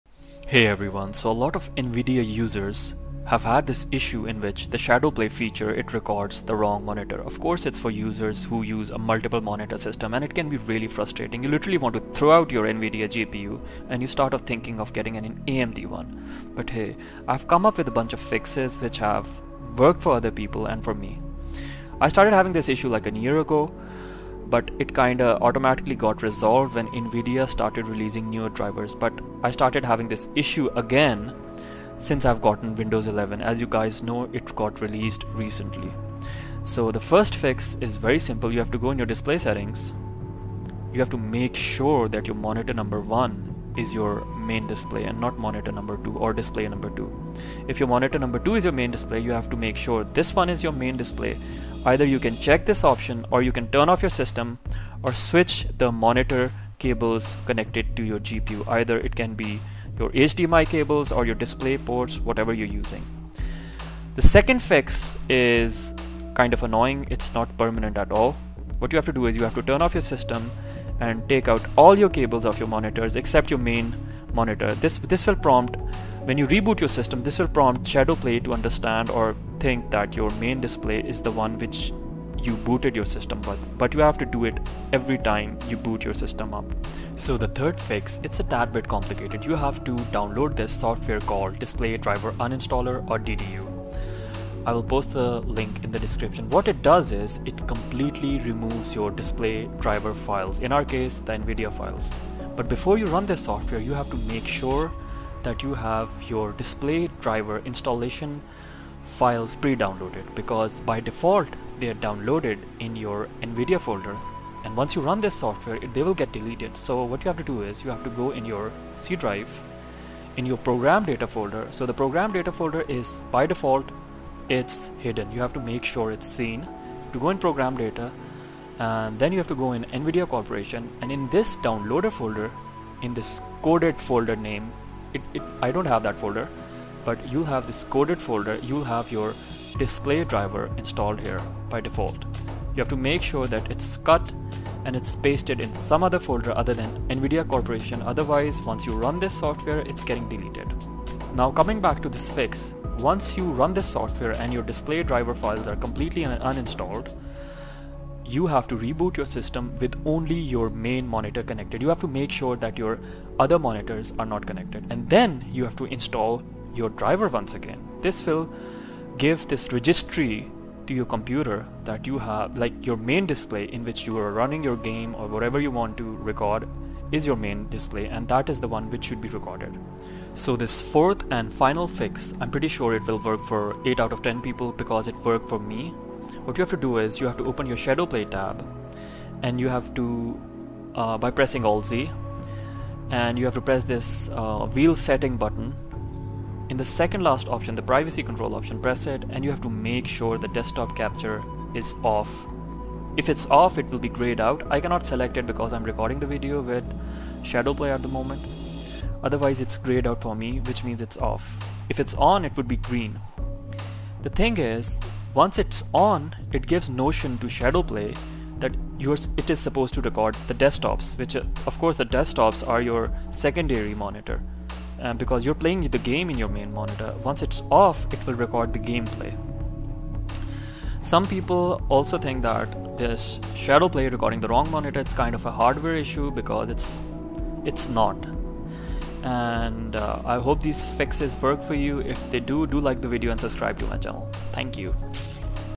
Nvidia Shadow Play Recording The Wrong Monitor | FIXED
A bunch of fixes to resolve the issue in which Nvidia ShadowPlay was recordeing or captured the wrong monitor. i started having this issue again once i got windows 11.

00:00 Introduction
00:36 First Fix
01:04 Second Fix
01:26 Third Fix
03:00 Fourth Fix